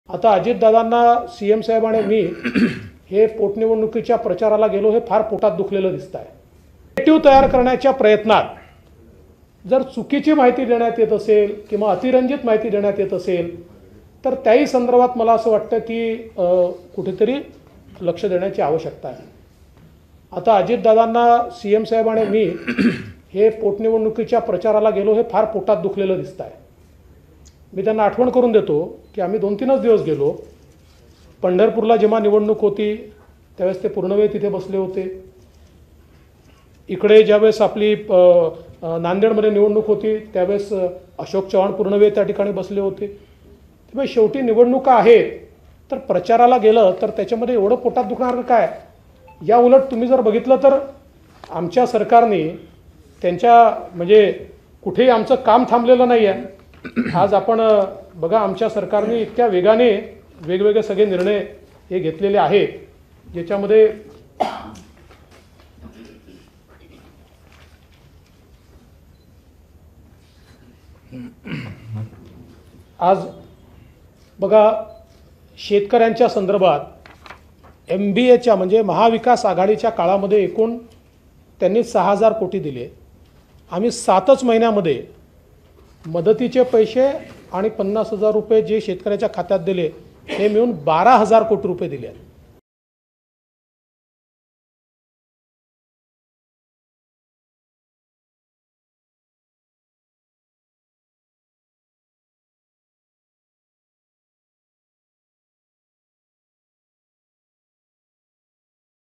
Devendra Fadnavis On Ajit Pawar : पोटनिवडणुकीच्या प्रचारावरुन फडणवीसांचा टोला | LetsUpp Marathi
पोटनिवडणुकीच्या प्रचारात आम्ही गेल्याने राज्याचे विरोधी पक्षनेते अजित पवार यांच्या पोटात दुखल्याचं खोचक विधान उपमुख्यंत्री देवेंद्र फडणवीस यांनी केलंय.